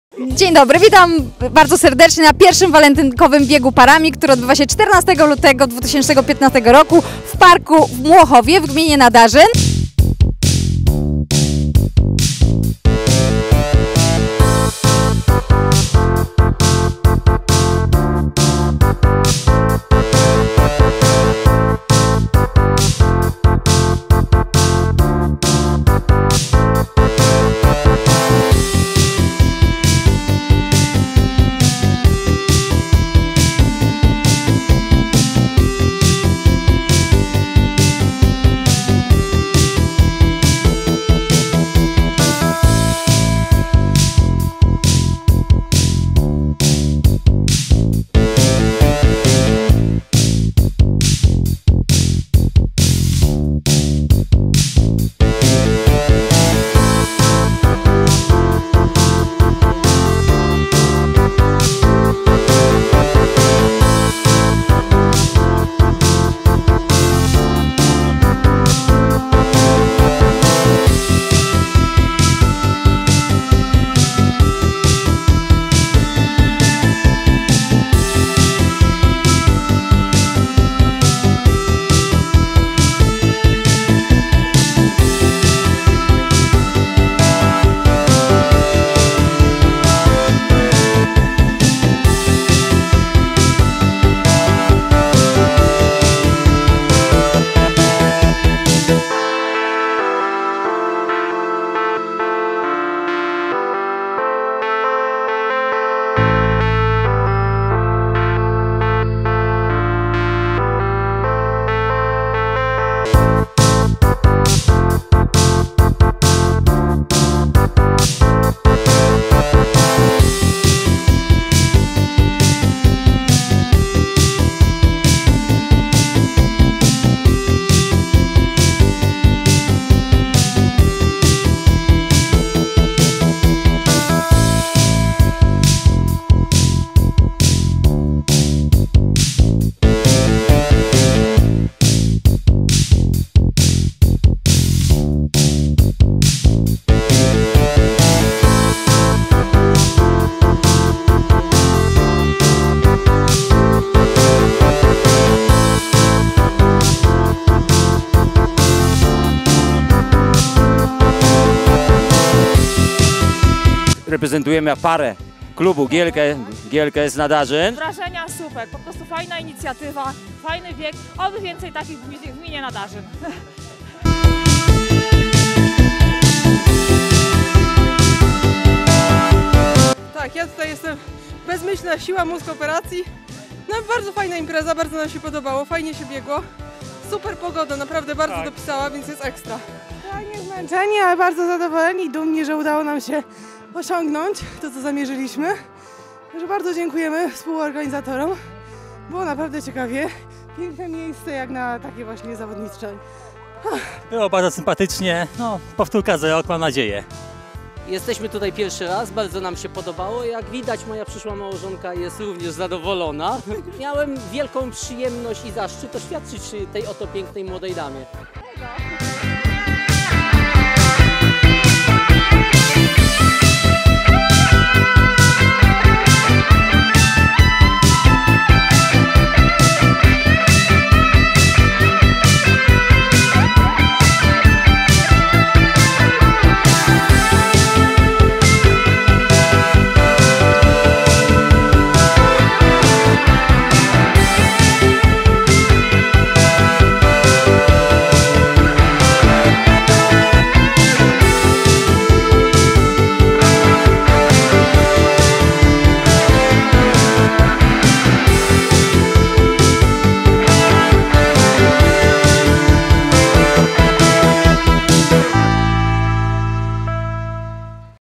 Bieg Walentynkowy
Pierwszy walentynkowy bieg parami w Młochowskim parku